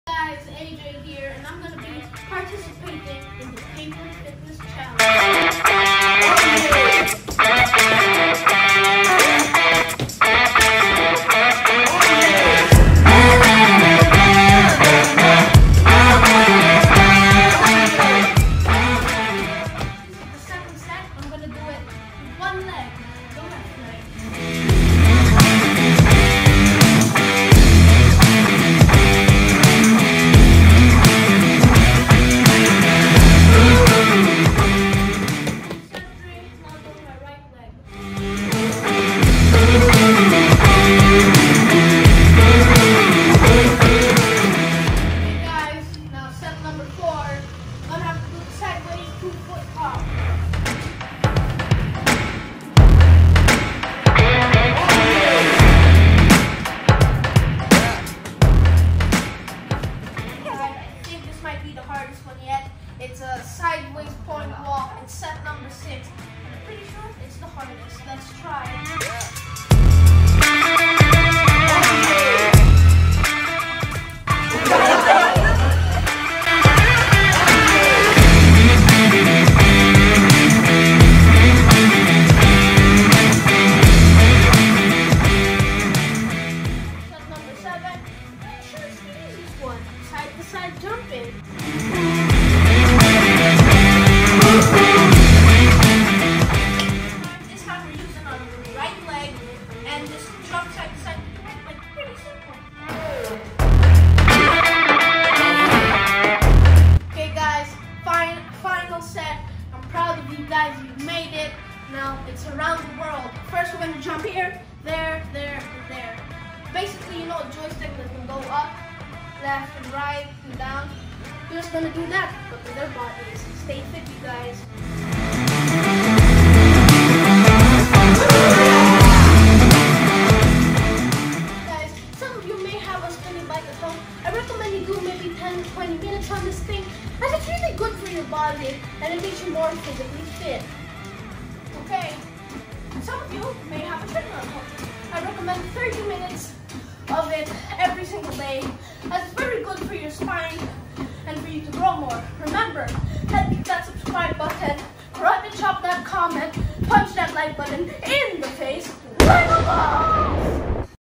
Easy workout for kids in quarantine
Paper fitness challenge for kids in quarantine